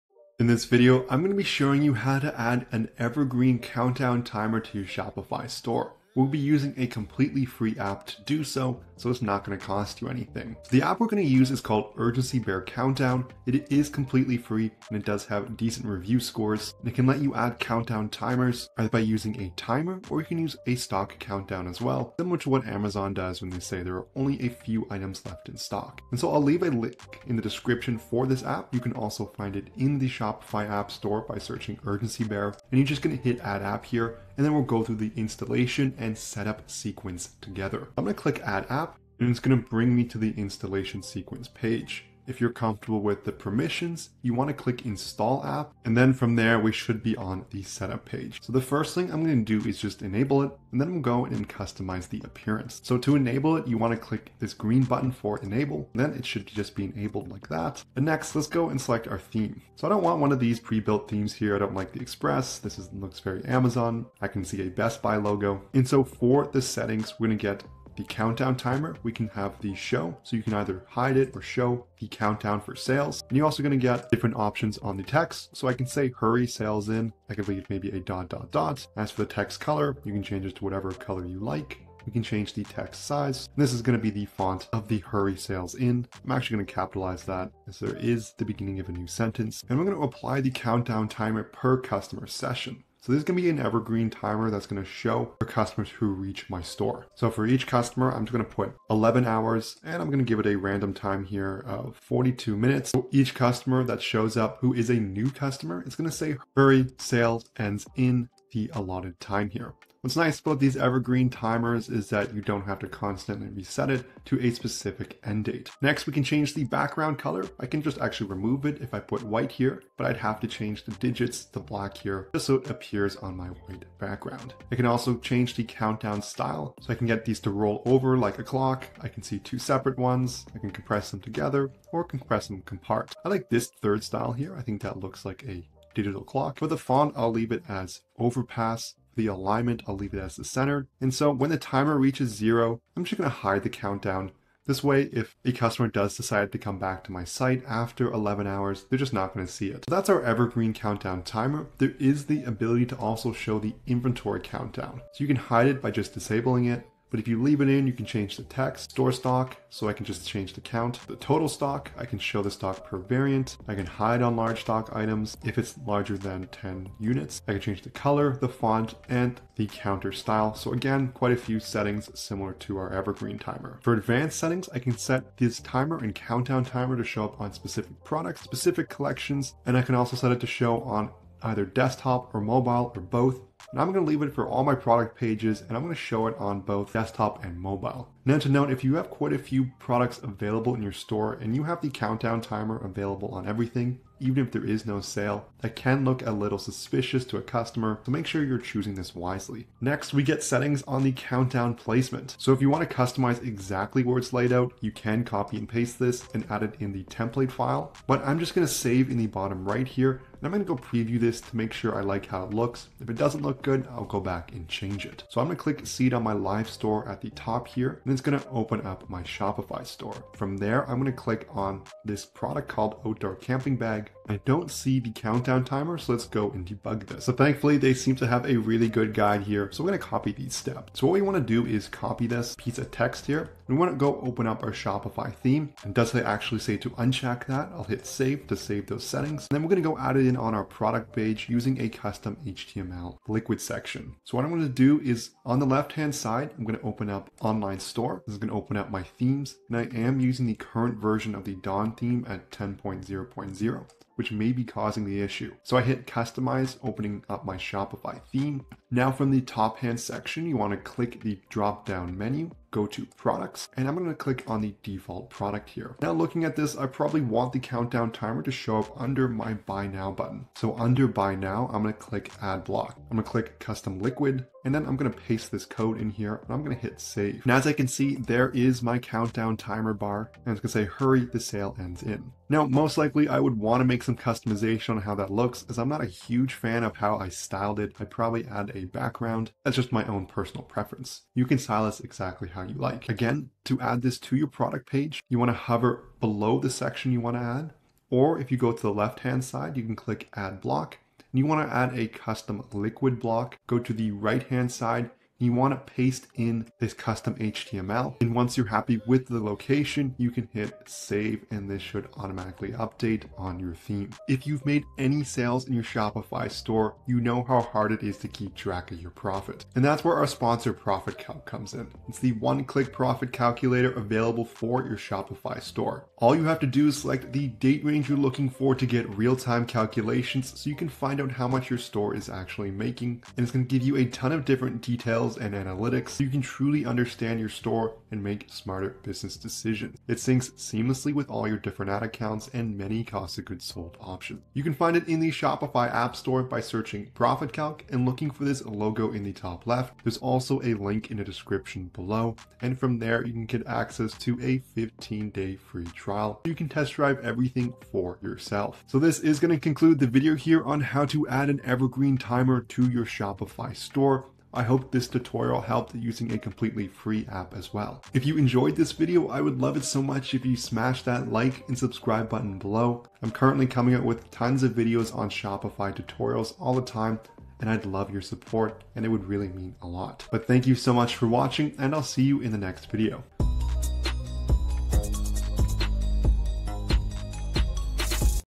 Evergreen Countdown Timer Shopify
Looking to create a sense of urgency and drive sales on your Shopify store? Look no further! In this video, we dive into the setup and features of the "Product Countdown Timer" app, an incredible free tool that will help you implement an evergreen countdown timer on your store.

With the "Product Countdown Timer" app, you can easily create and customize countdown timers for your products, creating a sense of urgency and motivating customers to make a purchase before time runs out. This app is perfect for limited-time offers, flash sales, and creating a buzz around your products.

In this video, we guide you step-by-step through the setup process of the "Product Countdown Timer" app. We cover everything from installation to configuration, allowing you to seamlessly integrate this powerful tool into your Shopify store.

Learn how to customize the appearance and position of your countdown timers to match your store's branding and design. Discover how to set the duration of the countdown and explore various options for displaying the timer on your product pages.

Whether you're a beginner or an experienced Shopify store owner, this video will provide you with all the information you need to leverage the "Product Countdown Timer" app effectively. Drive conversions, create a sense of urgency, and boost your sales with this must-have tool.

Don't miss out on this opportunity to take your Shopify store to the next level. Watch the video now and start implementing evergreen countdown timers to skyrocket your sales!

PROFIT CALC (Start your free 15-day trial)